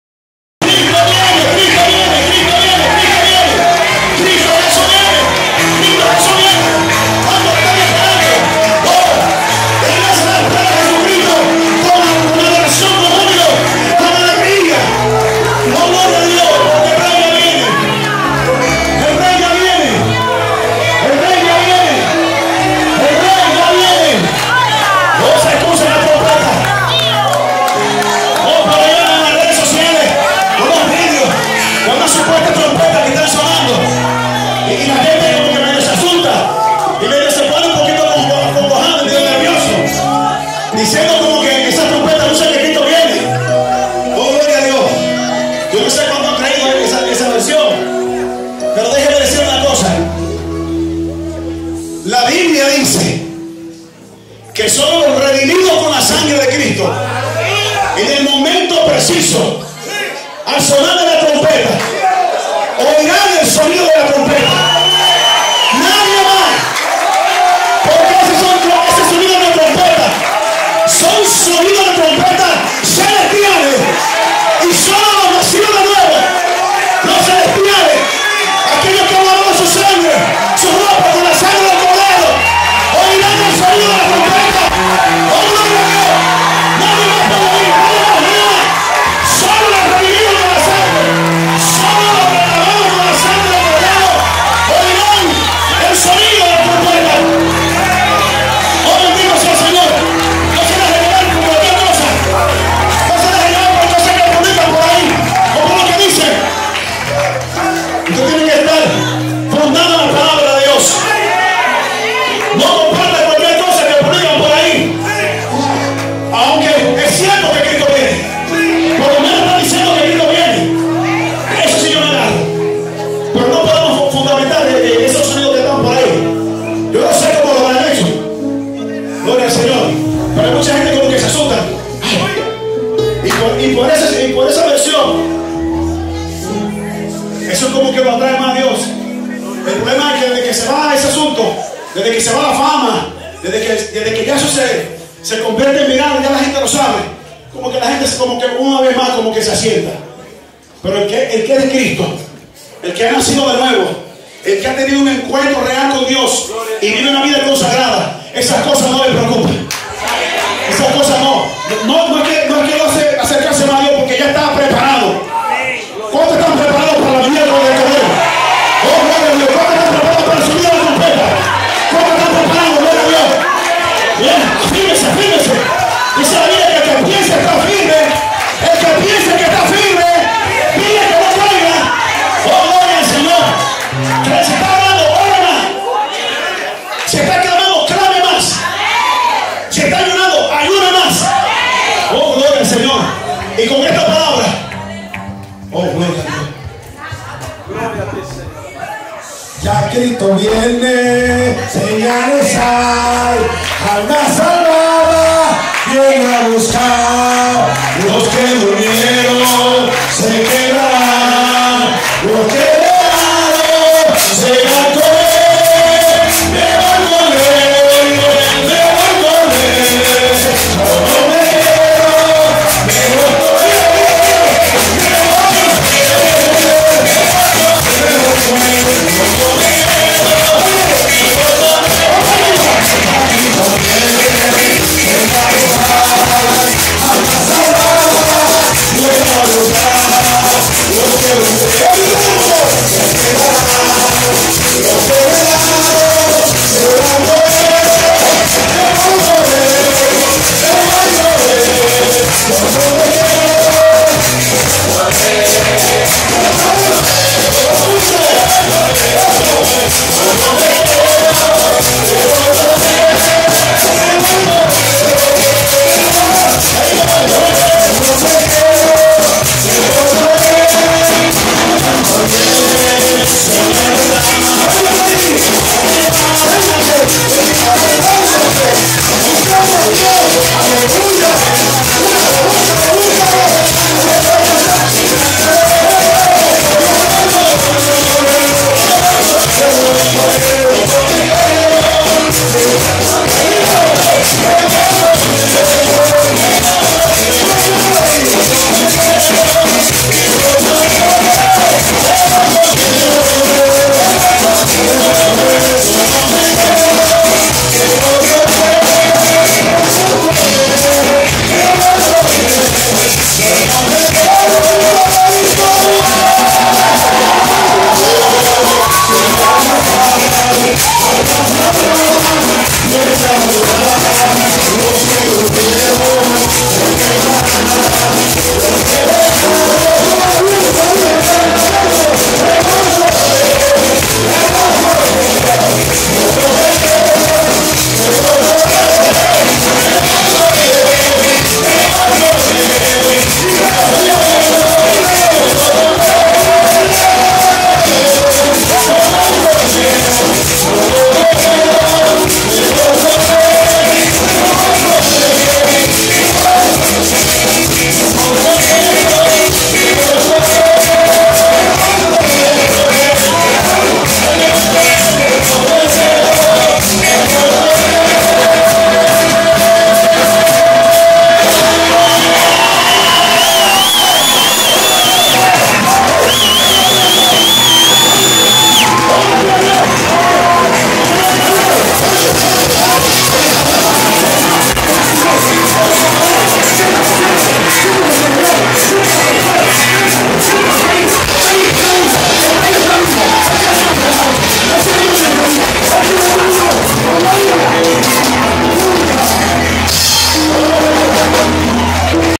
Omar Rodriguez y Ramón Arias, Cantando juntos un coro ll
Después de 40 días de ayuno el pastor Omar Rodríguez daba testimonio de la grandeza de Dios en su vida y en la vida de unos familiares que recibieron un milagro glorioso donde el  pastor Ramón Arias junto a él cantaban coros dándole la gloria al Señor de que él viene otra vez escuché con atención lo que Dios tiene que decirle 14/02/2016